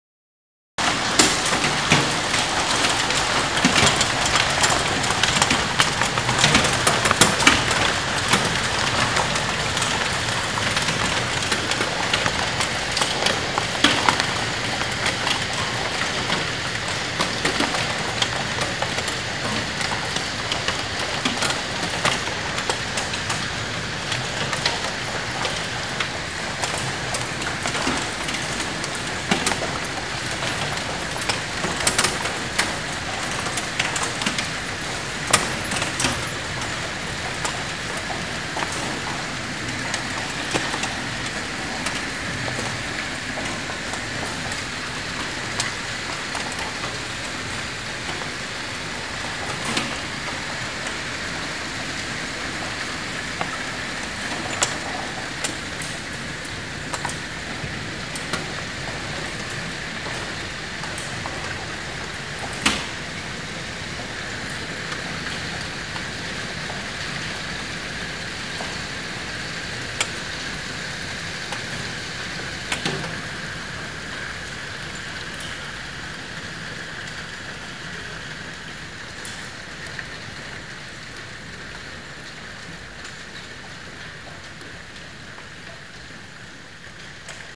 Primeras imágenes de la Impresionante granizada en Madrid
Granizada ocurrida el 3-7-14 a las 15,50 horas en la puerta de la escuela de CIne y Televisión Septima Ars en Madrid